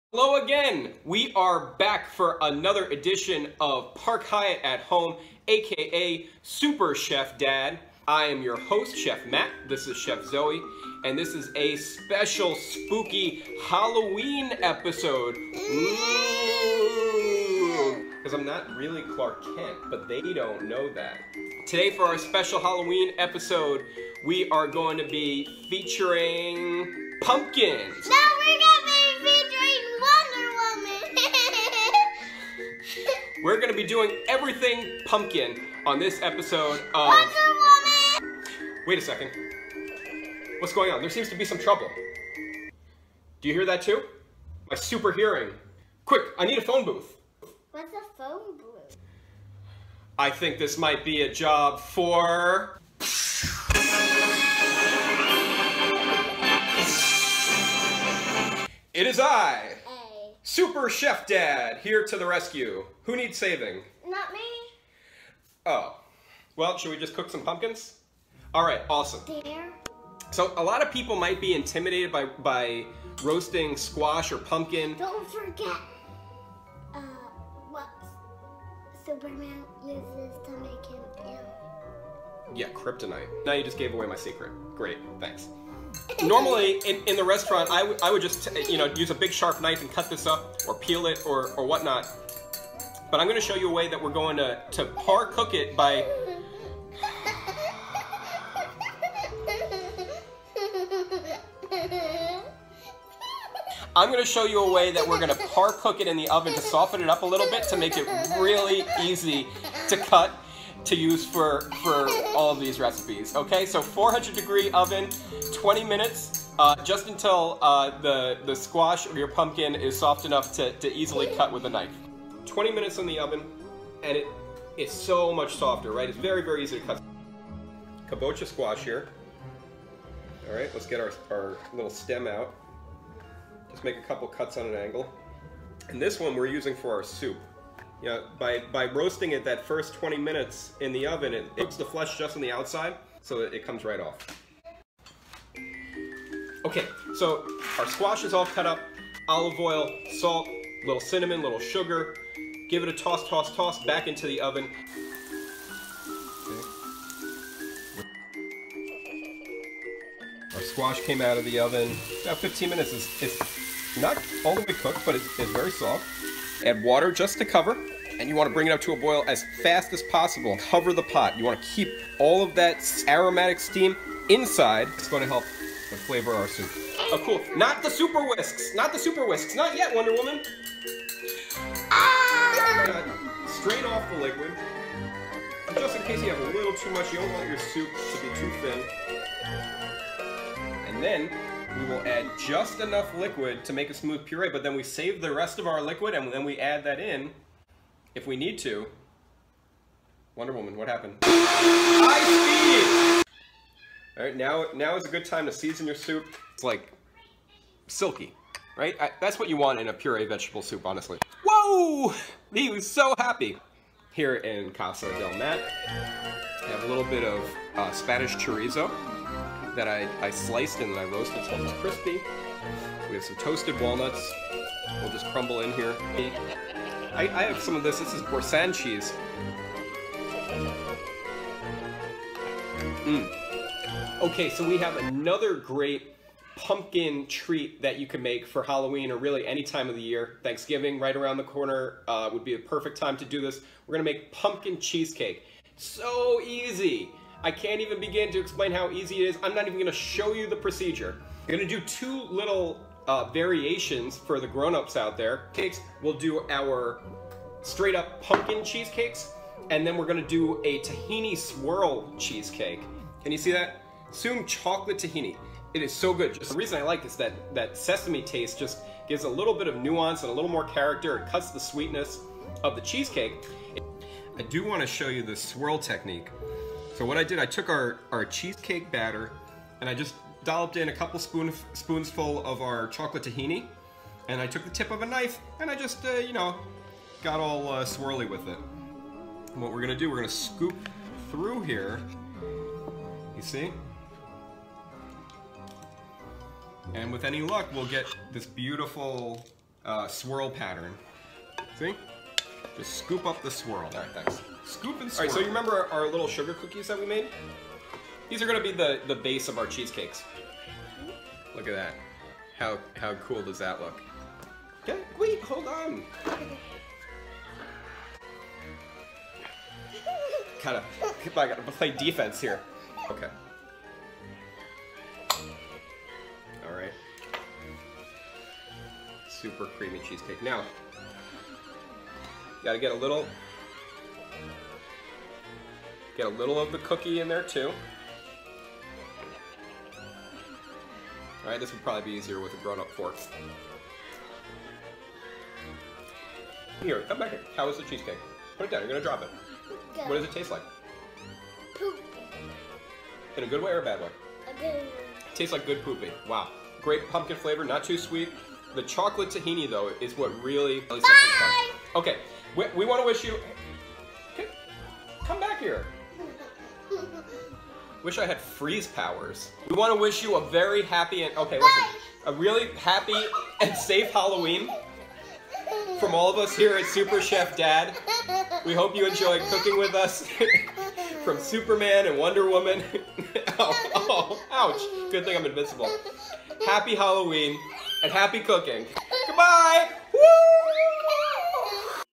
Super Chef Dad #26 - Halloween Pumpkin Recipes - Kabocha Squash Soup and Pumpkin Cheesecake
Happy Halloween! This week we are celebrating the great pumpkin (and squash in general).  In the video I’ll show you a delicious kabocha squash soup and chocolate tahini swirl pumpkin cheesecakes.  Below I’m also including recipes for an easy weeknight spaghetti squash side and how to toast all the seeds that come with our lovely squashes and pumpkins.
If you are really in a good mood, share us with your own friends and family.
Please message me with any requests you have for Thanksgiving recipes.  In the coming weeks I will devote each episode to a variety of classic sides and the bird itself.
Have a very happy and safe Halloween.  Love, Matt and Zoe

Kabocha Squash Soup

2# of Roasted Squash
1/2 a Large Yellow Onion, sliced
1 Large Shallot, sliced
3-4 Cloves of Garlic, halved
1/3-1/2 of a Large Carrot, approx. 1/4 C, sliced
1/2 Stalk of Celery, sliced
1/2 tsp Dried Thyme or 4-5 Sprigs Fresh
1/2 Stick of Butter, divided
Light Brown Sugar, to taste (optional)
Cinnamon or Nutmeg (optional)
Melt butter and add all vegetables
except for the squash.

Cook over medium high heat until the onions start to brown on the edges.  Season with salt and pepper.
Add roasted squash, herbs and spices and cook to caramelize further.  About 5 minutes.
Raise heat to high and add water to cover by 1/2".  Cover the pot and bring to a rapid boil.  Cook, stirring occasionally, until the squash is completely tender. Strain the soup and reserve all cooking liquid.  Transfer the solids to a blender.
Add just enough of the strained liquid to allow the blender to spin and puree the vegetables.  We don't want to add everything at once to ensure our soup doesn't become too thin.
BE CAREFUL WHEN BLENDING HOT SOUP!!!
Blend on the highest setting for 2 minutes.  In the last 30 seconds add remaining whole butter.  Season again to taste.

Pumpkin Cheesecake

12oz / 350g Cream Cheese (block not whipped)
10 oz / 290g Pumpkin Puree
1/2 C / 100g Sugar
1/2 tsp Ground Cinnamon
Pinch Ground Nutmeg
2 Large Eggs
1/3 C Chocolate Tahini (optional)
Heat oven to 325 and prepare a hot water bath in a pan large enough to hold the muffin tin.
Beat cream cheese until softened then whisk in pumpkin, sugar, spices and then the eggs one at a time.
Optional: Create swirl with tahini or simply mix into the batter.
Scoop into sprayed cupcake wrappers set in a muffin tin.
Bake 30 minutes or until just set.  Cool completely before removing from the pan.  Cool overnight before removing wrappers.

Spaghetti Squash

1 Spaghetti Squash
4 TBS Butter, Divided

Optional Ingredients:
Sweet: Maple Syrup or Brown Sugar, Ground Cinnamon, Cloves or Nutmeg
Savory: Dried or Fresh Thyme, Finely chopped Garlic, Grated Parmesan

Cut the squash in half and remove seeds.  Place cut side up in a glass baking dish or other microwave safe vessel with sides of at least 2-3”.
Cut half of the butter into cubes and place in the cavity of the squash along with a seasoning of salt and pepper and, if desired, sugar or syrup and spices.
Add water to the pan around the squash to reach ½” then wrap tightly in a double layer of plastic wrap.  Microwave 10 minutes and check to see if the squash is tender by piercing it with the tip of a small knife.  If it is still hard, rewrap if necessary and continue cooking in two minute increments until completely tender.
Remove plastic and cool to room temperature.  Use a food to remove the flesh from the skin of the squash.  It will naturally shred into long spaghetti like strands.
Gently stir in remaining butter and any optional additional ingredients.  Cooked squash can be frozen in resealable freezer bags for up to 1 month.

Roasted Pumpkin Seeds

Place seeds in a large bowl and cover with cold water.   Rub the bundles of seeds together to remove the fibrous membrane that holds them together.  The pulp should fall to the bottom of the bowl while the seeds float.  Take care to remove all of the orange pulp completely.  Drain and dry on paper towels

Heat oven to 210 F.  Place seeds on a sheet pan in one later and bake 15-20 minutes or until dry to the touch but not toasted.   Set aside to cool slightly then transfer to a clean bowl.

Raise oven temperature to 325 F.  Toss every 2 cups of seeds with 2 tsp Olive Oil, Salt to taste and any additional flavoring (suggestions below).  Spread back out on the sheet pan and roast, stirring occasionally, until golden brown and crisp, about 20 minutes.  Cool and then store in an airtight container.

Flavor Options

Cinnamon Sugar
Honey, Soy Sauce and sesame Oil
Garlic, Rosemary and Parmesan
Cumin, Chili Powder and Cayenne Pepper
Curry Powder and Shredded Coconut

Thanks to BenSounds.com for a portion of the audio content.